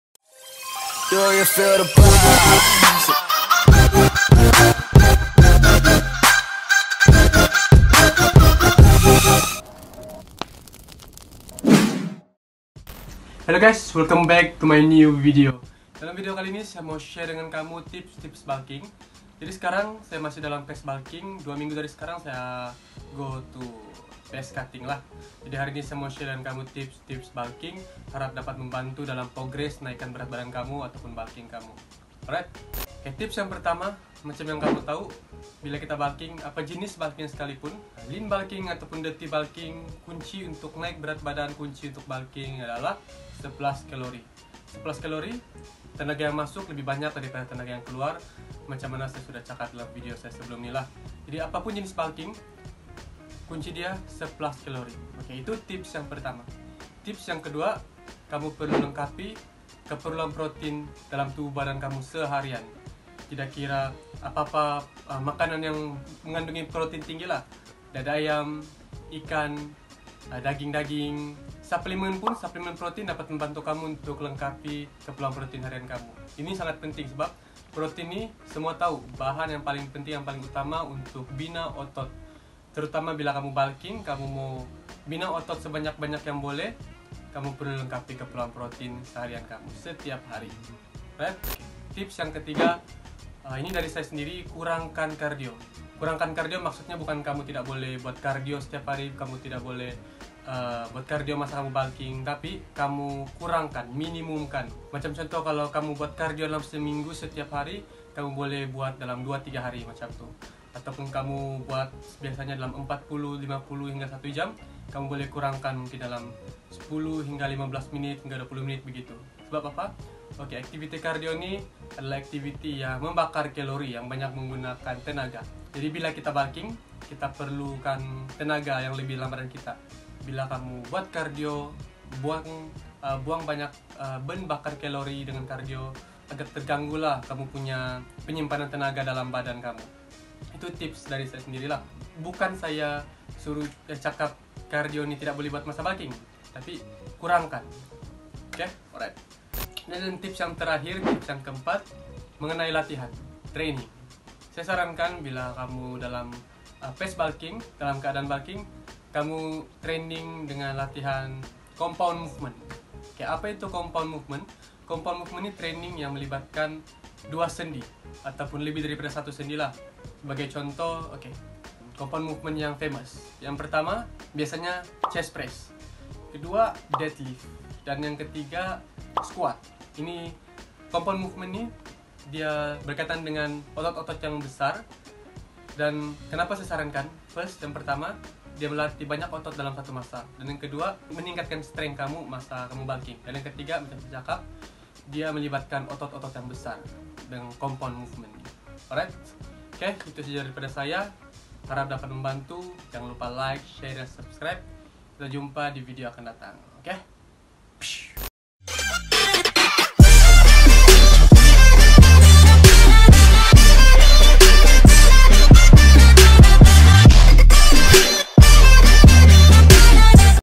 Tips Bulking / Naik Berat Badan - JreeyJorosemy
Helo gais , video kali ni berkaitan dengan Tips Bulking atau naik berat badan . Harap Tips yg sy share skit sbanyak dpt bantu kamu dlm progress bulking kamu.